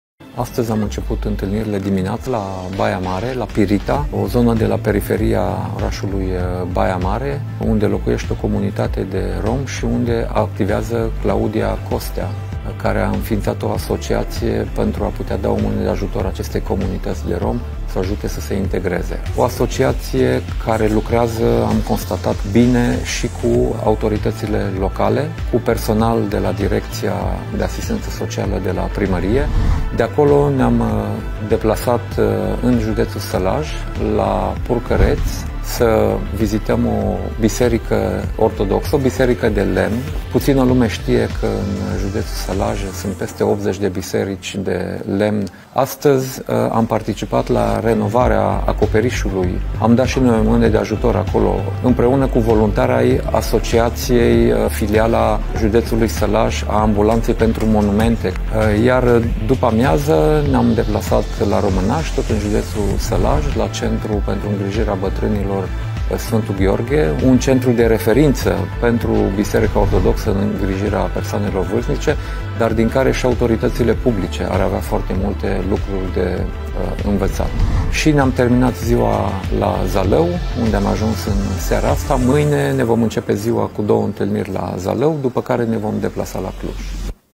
Caravana RomâniaEuropeană cu Dacian Cioloș - 26 aprilie
Dacian Cioloș a decis să ia România la pas, să meargă în toate regiunile ei pentru a descoperi proiecte, idei și inițiative materializate în reușite care, dacă sunt cunoscute și sprijinite suficient, pot transforma România în țara în care să trăim mai bine. O Românie europeană, împlinită, de care să ne mândrim în casele noastre, în suflet, nu pe care să o revendicăm la televizor și în tribune. România oamenilor buni, a celor care o construiesc.

Miercuri, 26 aprilie, Dacian Cioloș a început ziua cu o vizită în comunitatea Pirita — o comunitate marginalizată din Baia Mare care, alături de Asociația Pirita Children, promovează accesul la educație și un trai decent pentru copiii din Pirita.

Mai apoi a dat o mână de ajutor în intervenția de urgență la biserica de lemn din Purcăreț, alături de Asociația Arhaic, în vederea consolidării unui monument istoric, și discuții privind activitățile și planurile lor de viitor.

Urmată de o vizită la Centrul de Servicii Sociale „Sf. Gheorghe”: centru rezidențial pentru persoane vârstnice organizat sub egida Episcopiei Sălajului.